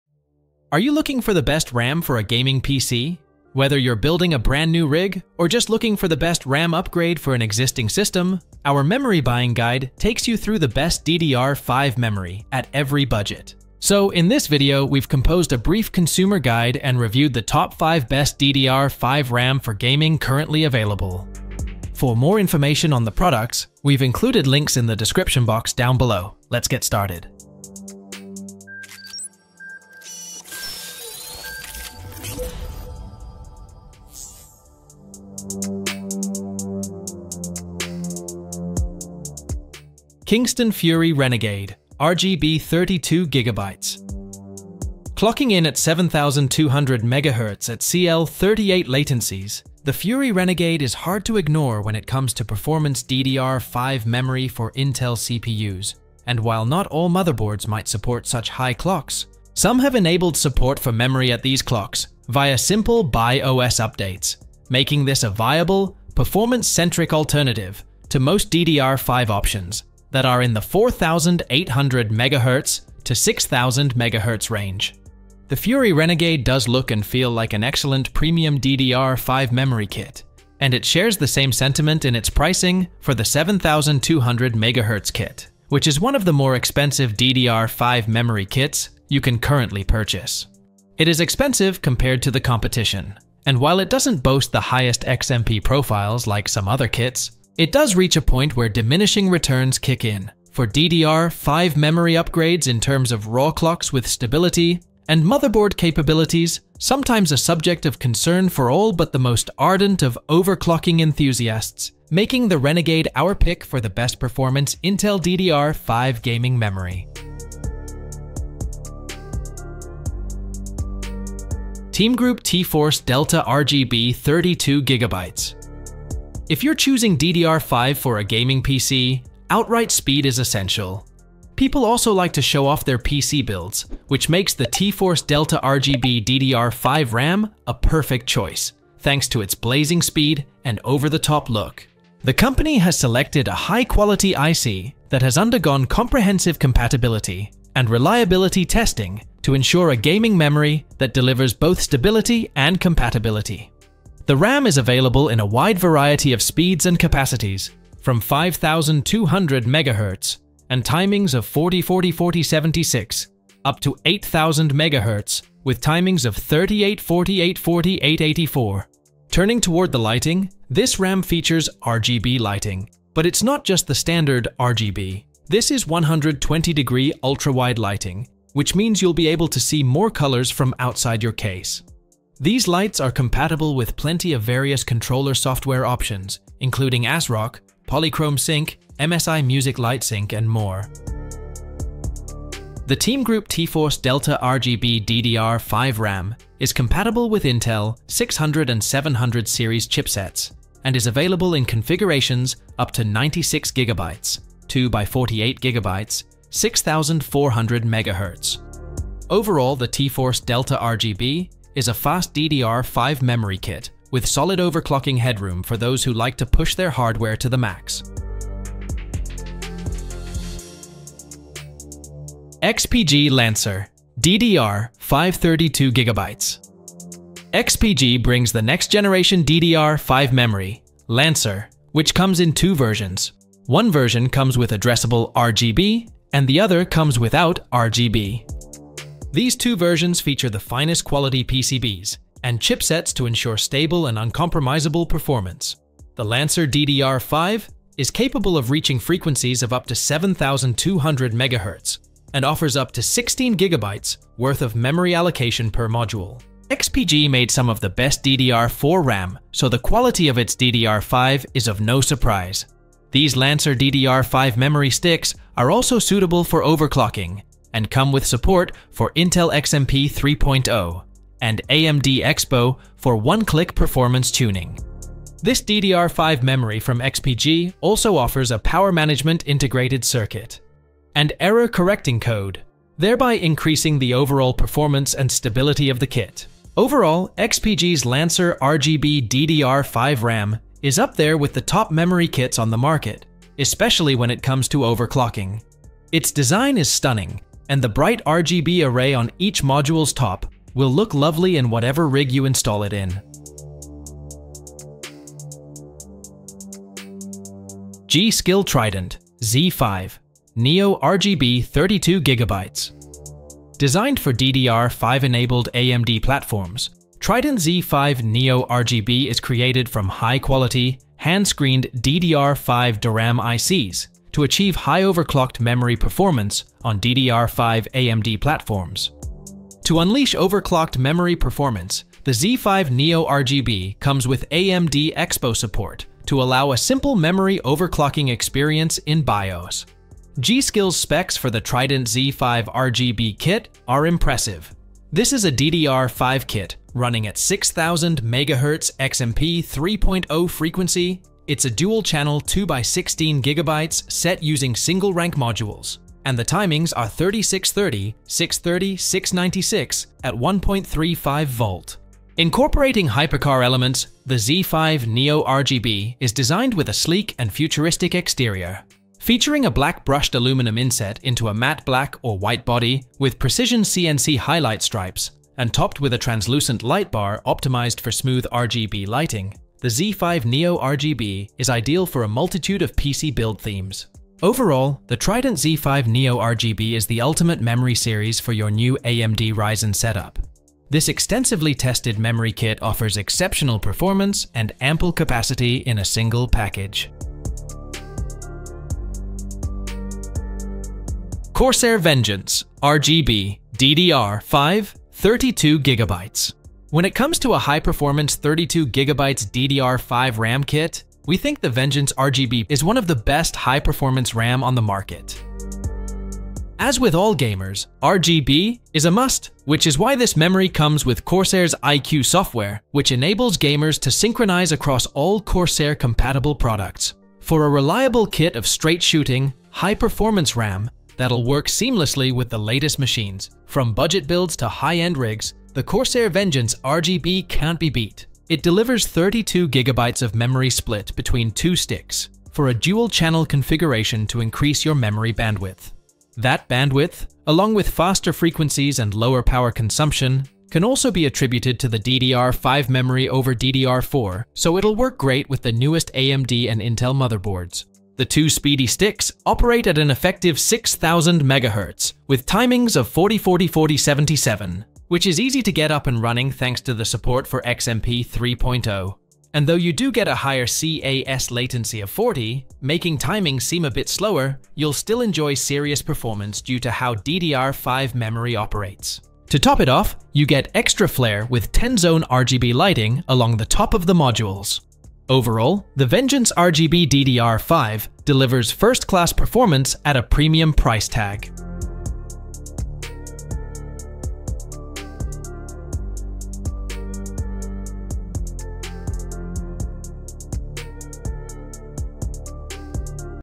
Best RAM for Gaming - Top 5 Best DDR5 RAM for Gaming you Should Buy in 2024
Top 5 Best DDR5 RAM for Gaming you Should Buy in 2024 !
[Purchase Link] ↧↧
➜ 5. Kingston Fury Renegade RGB :

►Are you looking for the best RAM for a gaming PC? Whether you’re building a brand new rig, or just looking for the best RAM upgrade for an existing system, our memory buying guide takes you through both the best DDR4 memory and the best DDR5 memory, at every budget. So, in this video, we’ve composed a brief consumer guide and reviewed the Top 5 best RAM for Gaming currently available.